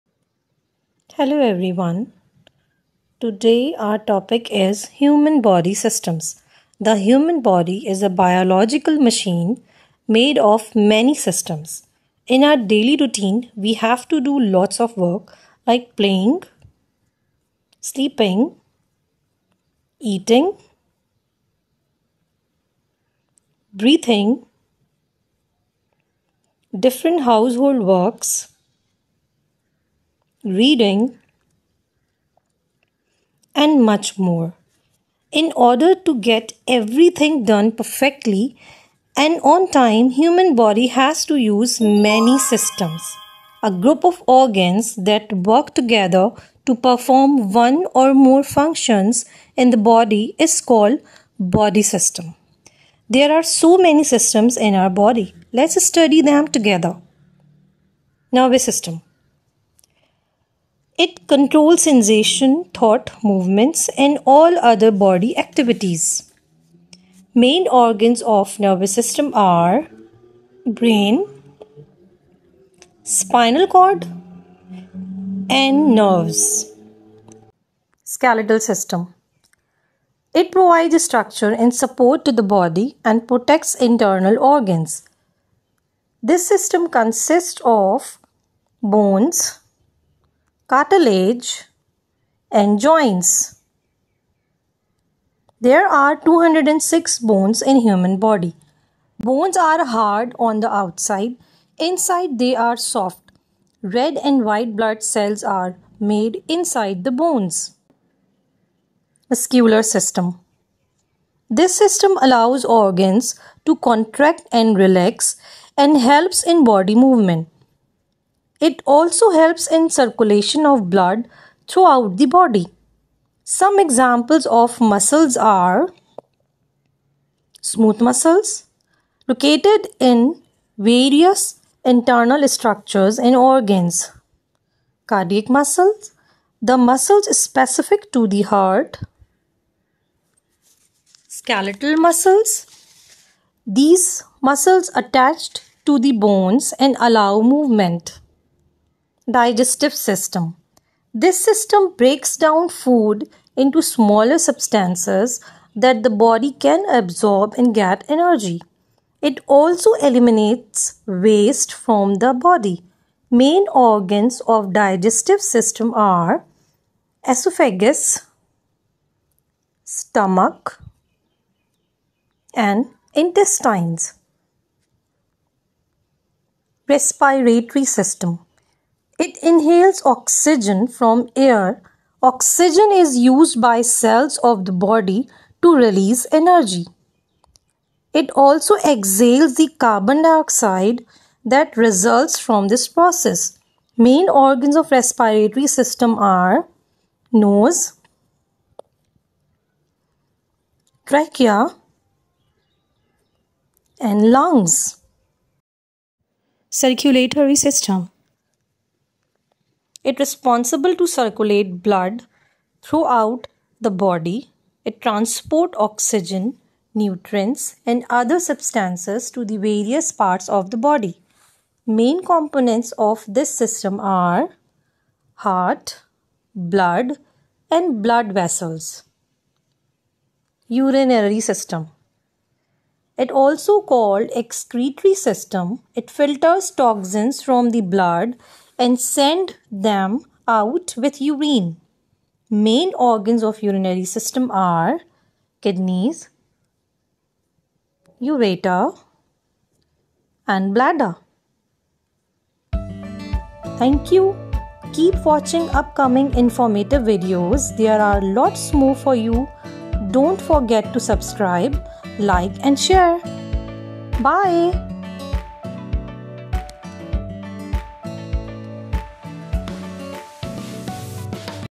Human Body Organ Systems and Functions | 7 important human body systems
In this video you will learn about some important human body organ systems and their main functions

Important human body systems ,
human body functions overview
animated video
educational video
science video